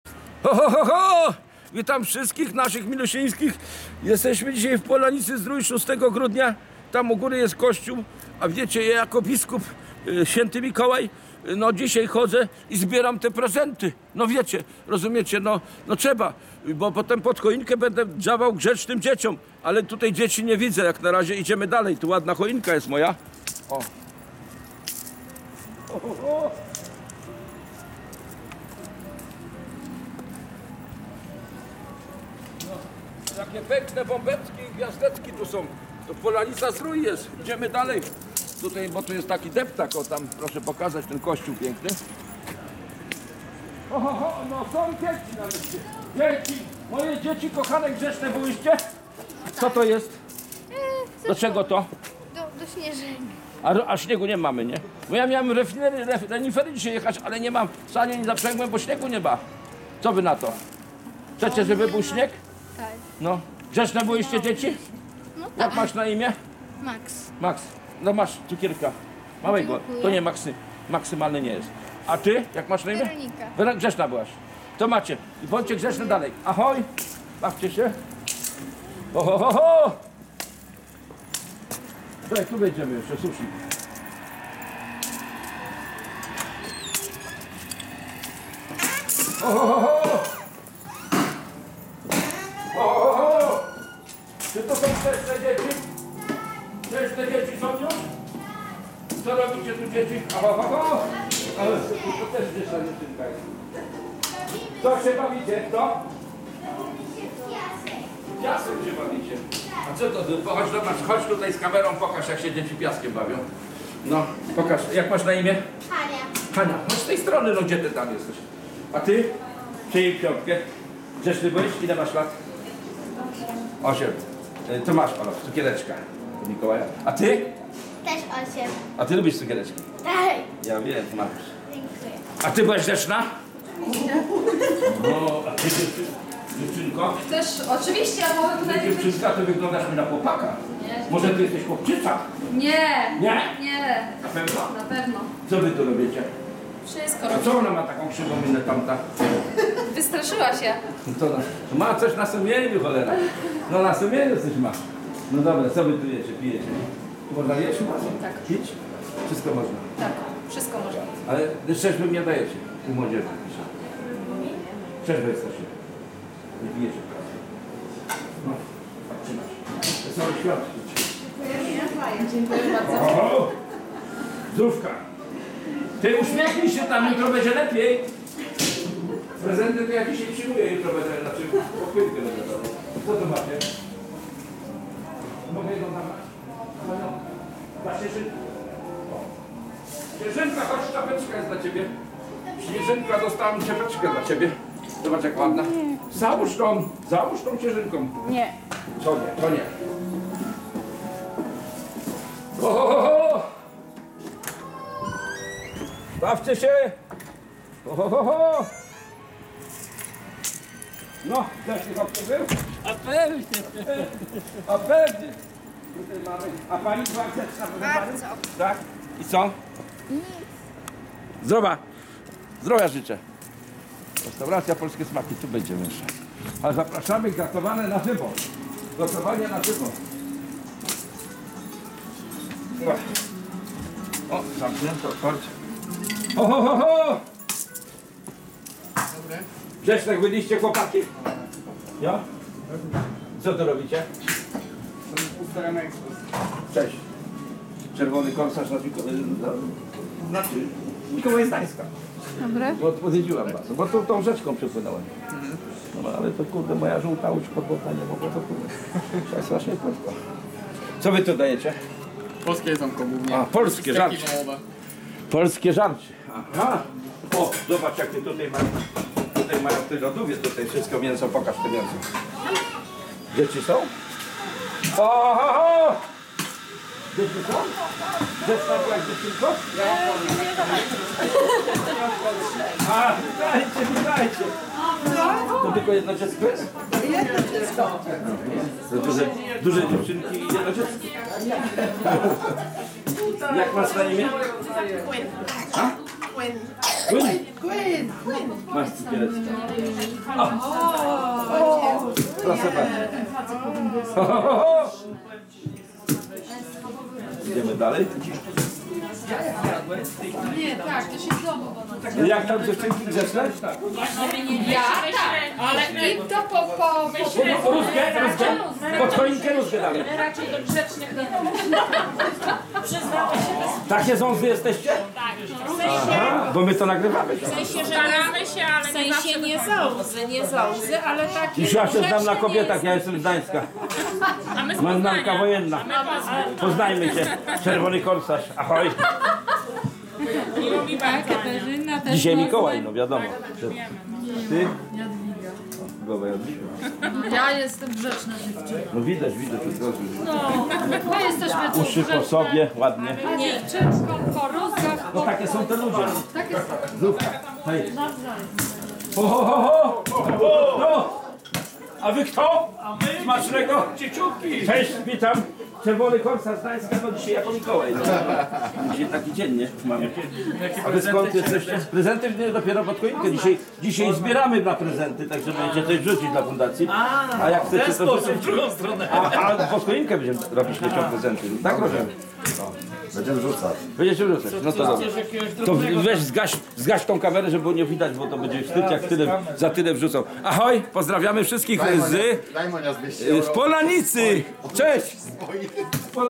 POLANICA -ZDRÓJ IŚWIĘTY MIKOŁAJ PROSTO Z GDAŃSKA ROZDAJE CUKIERKI DZIECIOM NA DEPTAKU I RESTURACJI
W Polanicy Zdrój jest urokliwe miejsce ,  gdzie można odpocząć w spokoju i ciszy bez telewizora i radia przy akompaniamencie Koguta , kaczki i gęsi...Jest dawna rezydencja sekretarza Magistratu zbudowana w 1928  roku. Warto tam zawitać i się zadomowić i można sobie wybrać siu się stamtąd do na Stoki narciarskie  Zieleńcu a także pojeździć na górskim rowerze. Pieszy też znajdzie wiele tras do pokonania z pięknymi widokami...ZAPRASZAMY